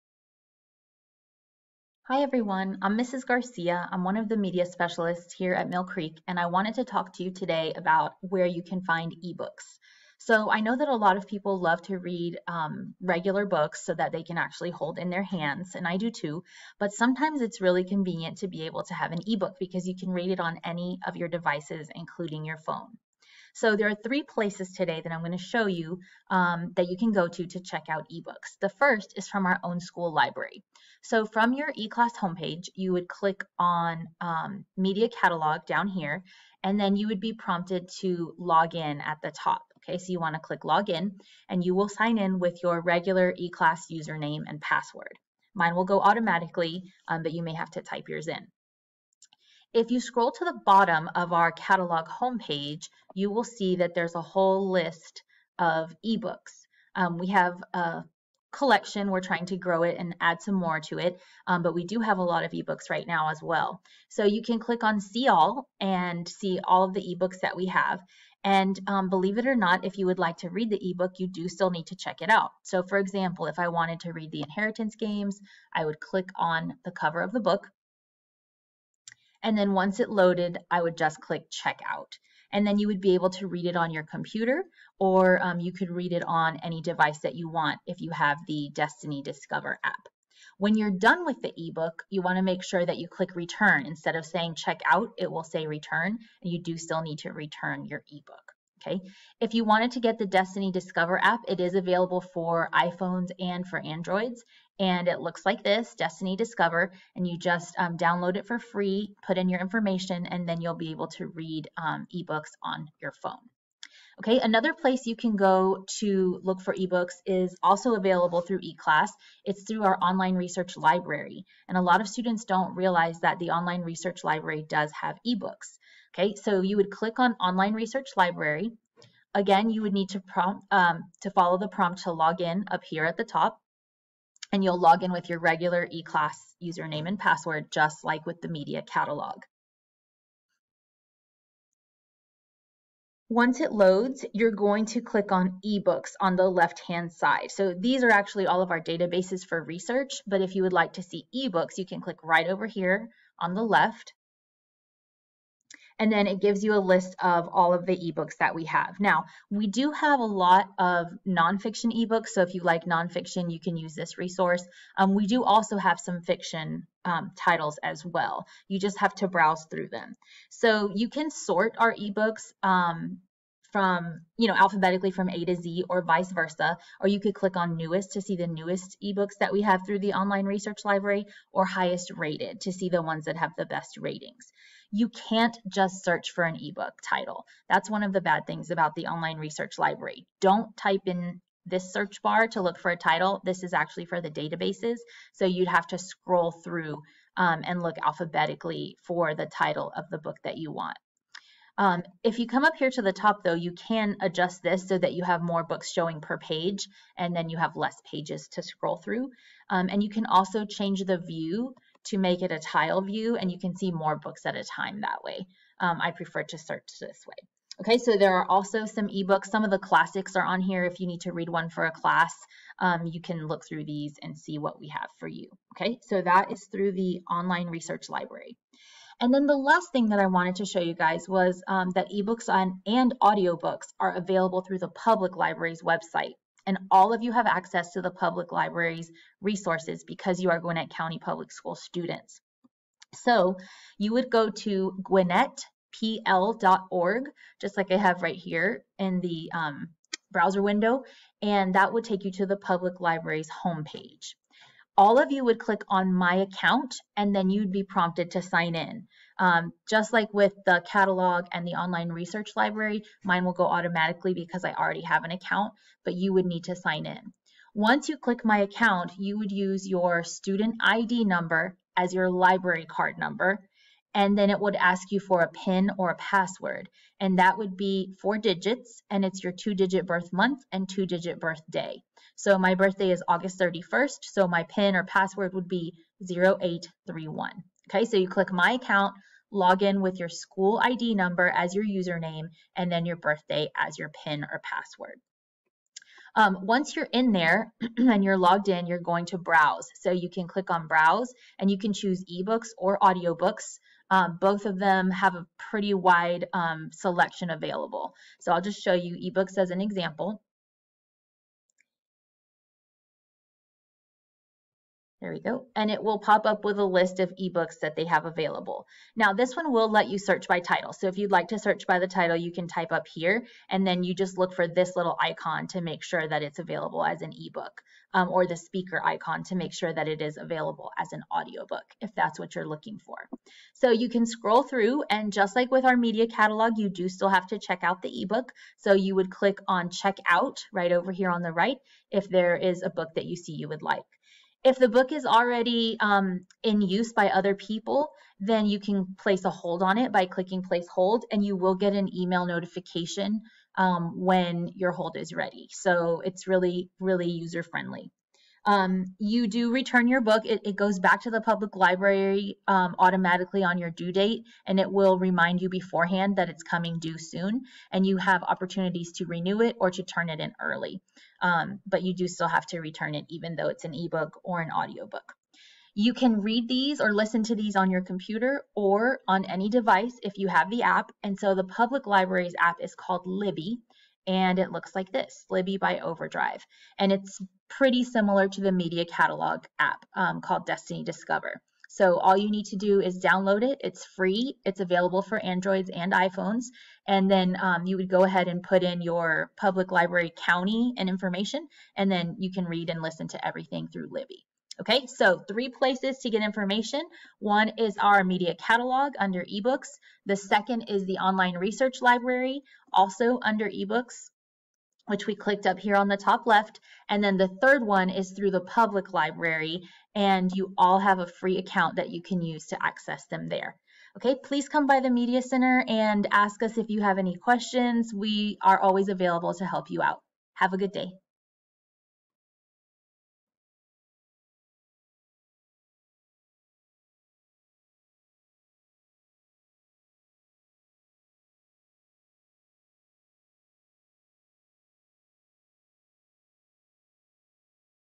eBooks
How to access ebooks and audiobooks
1. Destiny Discover
2. Online Research Library
3. Gwinnett County Public Library